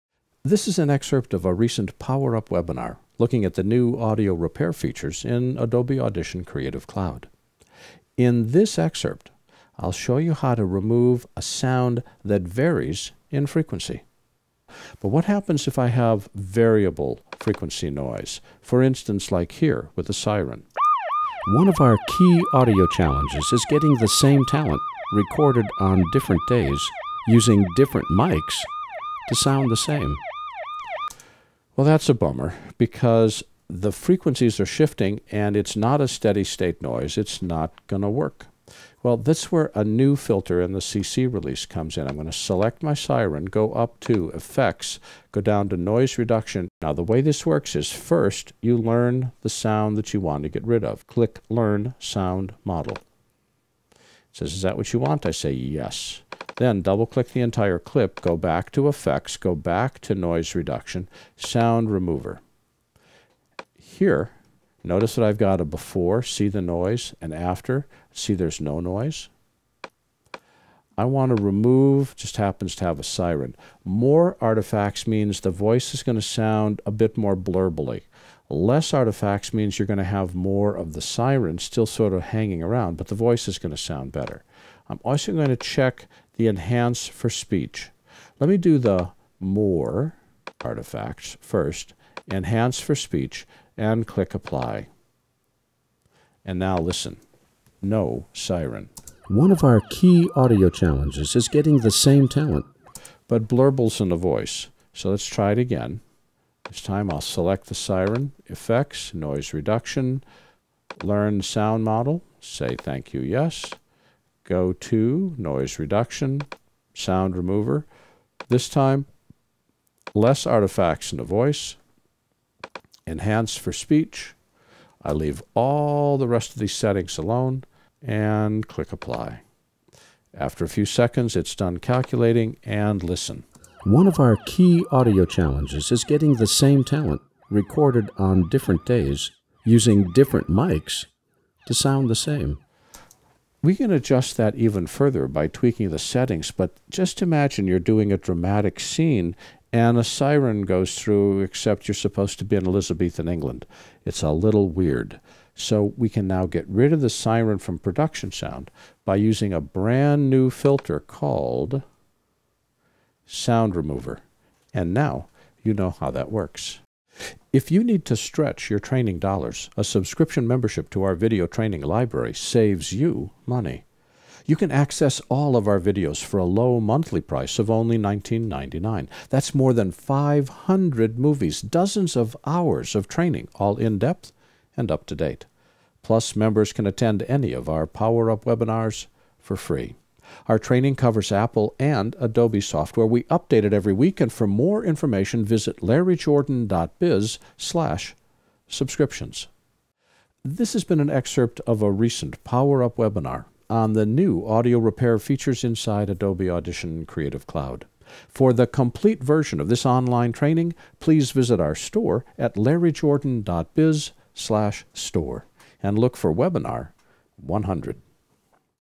Remove Sound That Varies in Frequency
- How do you remove sound that varies in frequency? How do you fix sound recorded on different days?  Larry's got the answer for you in this video.  To get the complete webinar "Repair Bad Audio in Adobe Audition CC," - Enjoy!